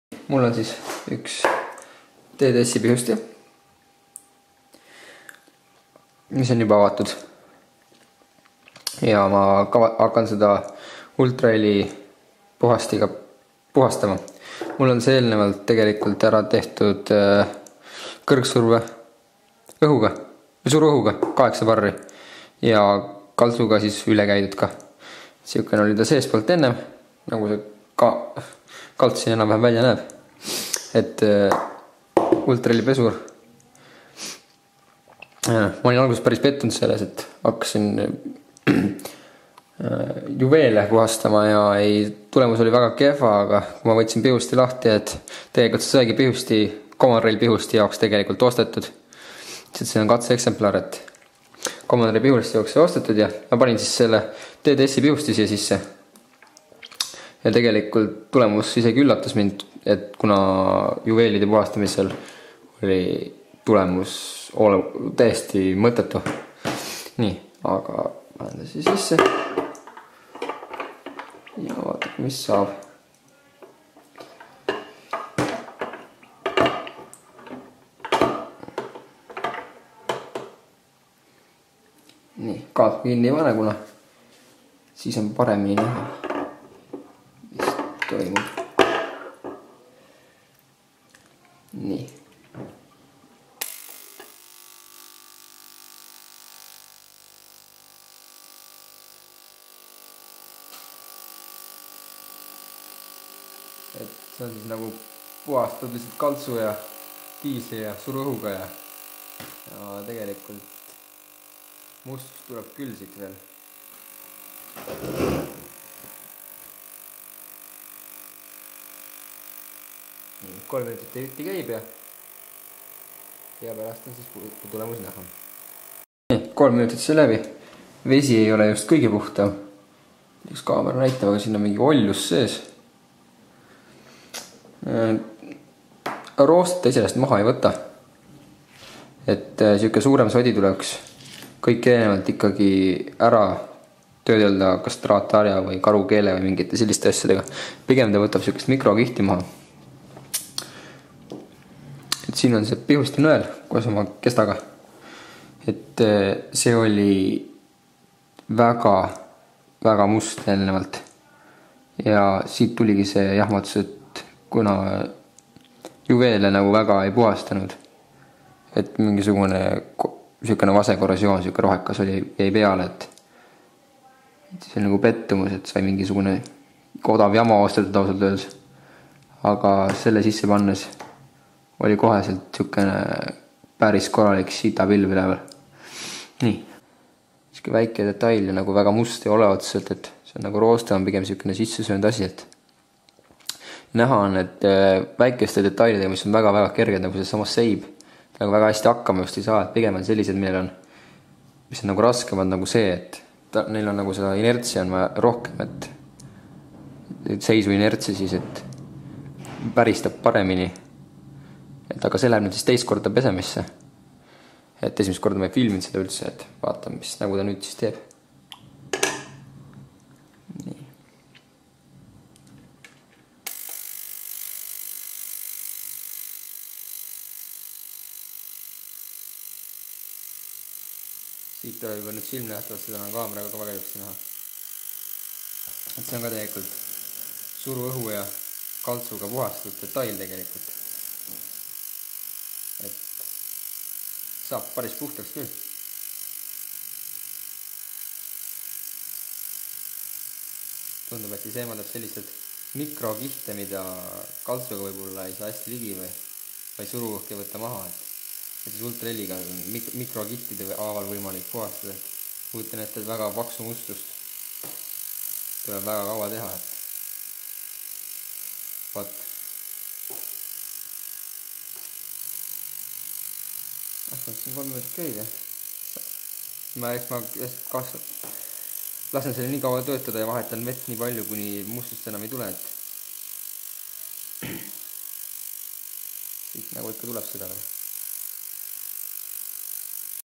Pihusti ultraheli puhastus
Pihusti täielikuks ja audentseks kontrolliks on see lisaks vaja ka stendida selleks ettenähtud pingil, mis on oluliselt kallim, kui videos nähtav ultraheli puhastus seade.